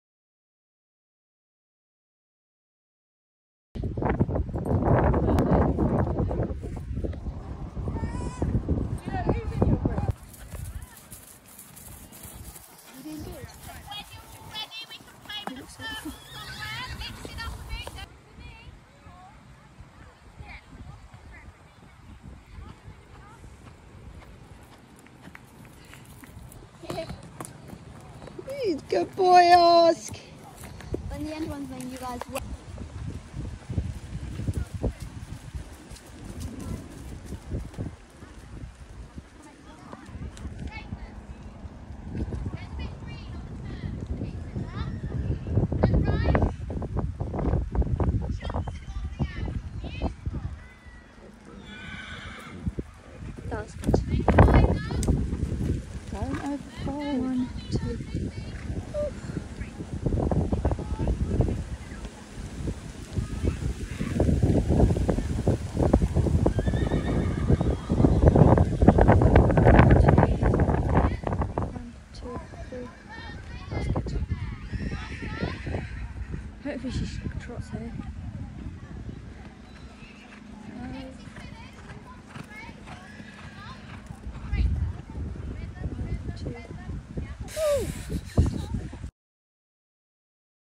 First wet day at pony club camp. We didn’t have studs so we took it really carefully.
Oscar you are totally brilliant, he was amazing . We couldn’t love this pony anymore, he is just everything we dreamt of. It was wet and horrid but we got through it, looking forward to tomorrow.